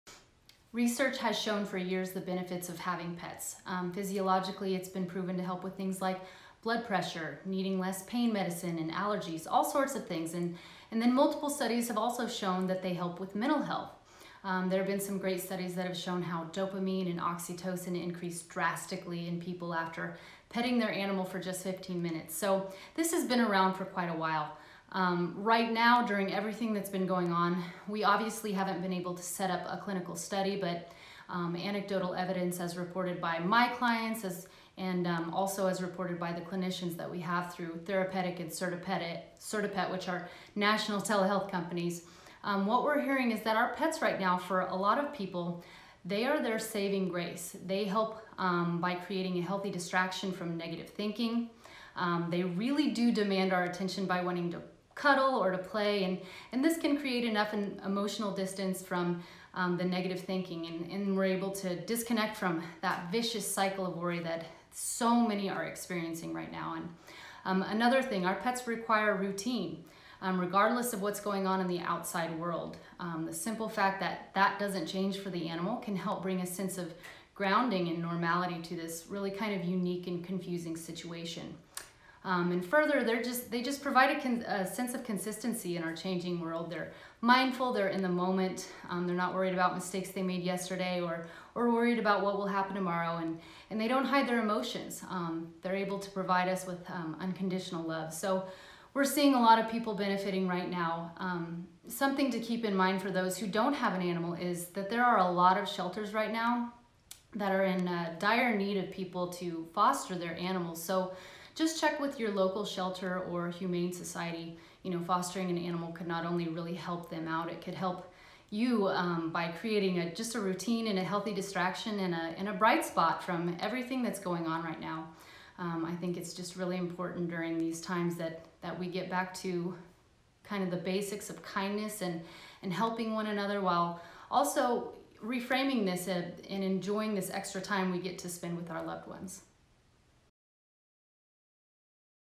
How Your Pet Helps You Cope With Stress: A Therapist Weighs In
Licensed mental health professional, Prairie Conlon, weighs in on the stress you may be feeling during COVID-19 and how your pet may be your saving grace.